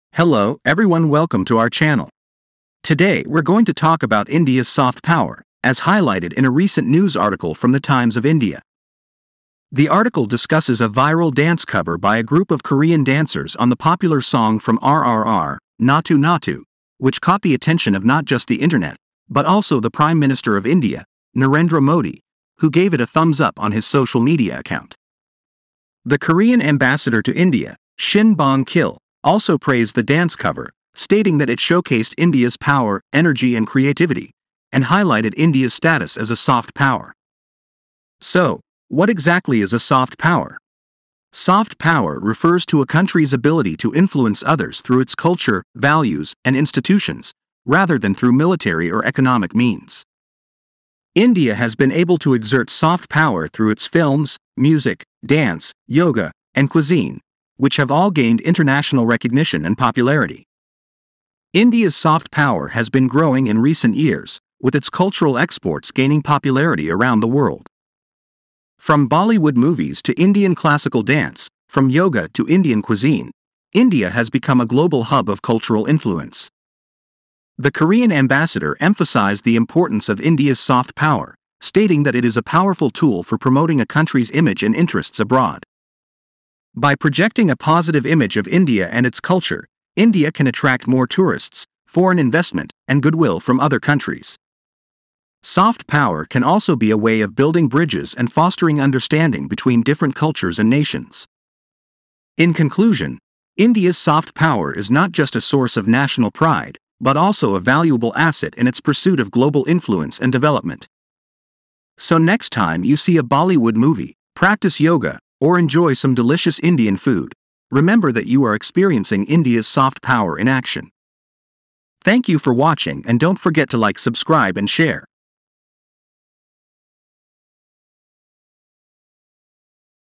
India's Soft Power: How Culture Shapes Influence
Discover how India's cultural exports, including films, music, dance, yoga, and cuisine, have helped to establish it as a global hub of soft power.

 From a viral dance cover on a K-Pop song to Bollywood movies, Indian classical dance, and cuisine, India's soft power is becoming increasingly influential around the world.

Learn about the significance of soft power for promoting a country's image and interests abroad, attracting foreign investment and building bridges between different cultures and nations.